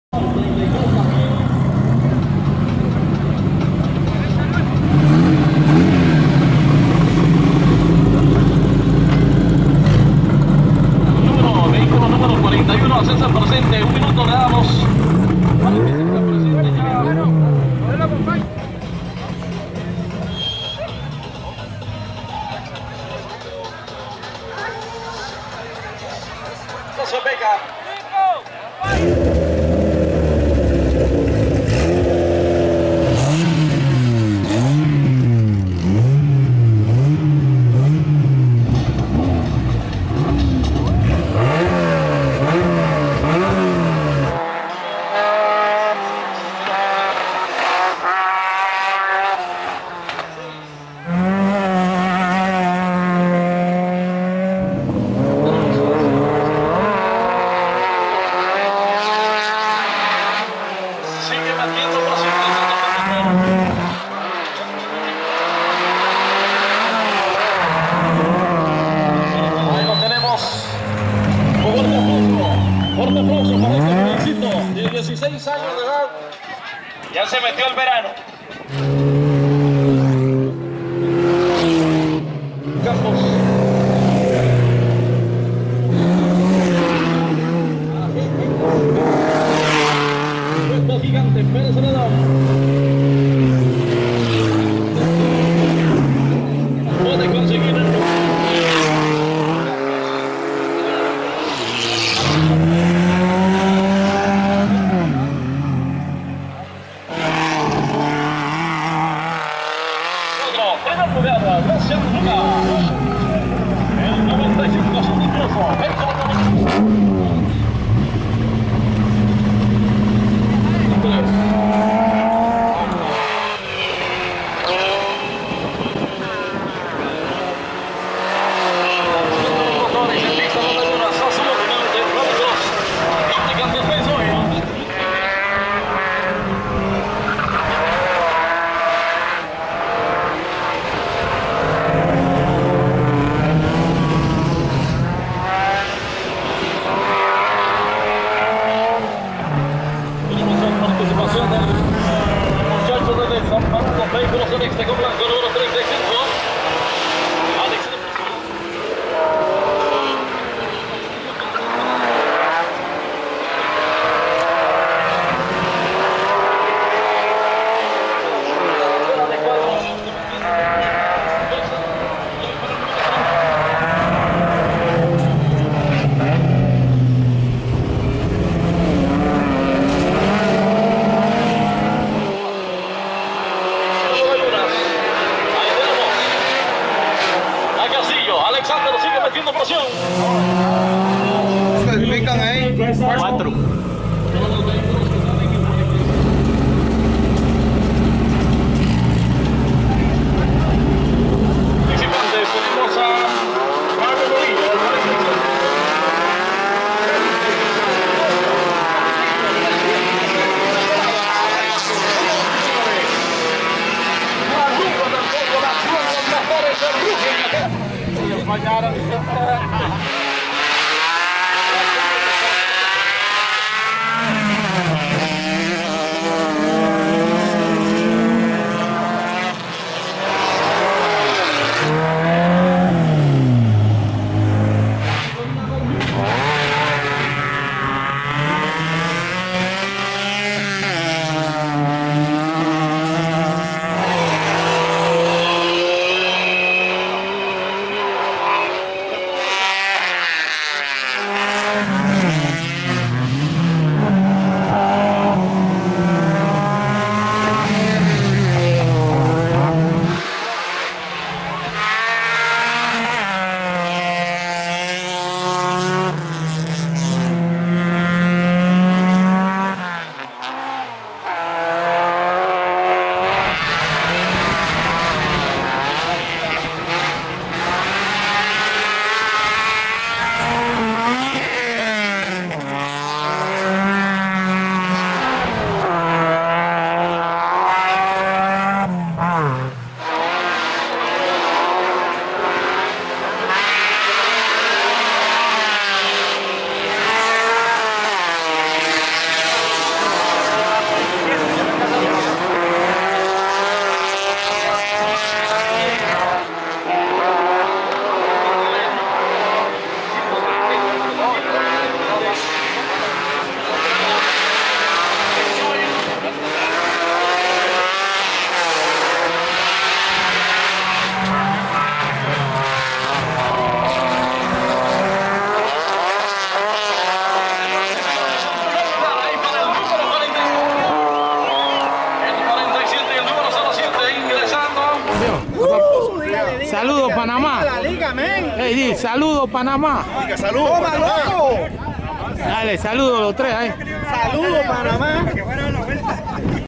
TBT RALLY CROSS - PEREZ ZELEDÓN
Carreras de Rally Cross grabada por Bandera Kuadros en sus inicios...visita ala calida ciudad de Perez Zeledón en compañía de nuestro hermano Edwin Carvajal...buenos momentos recavados por nuestra camara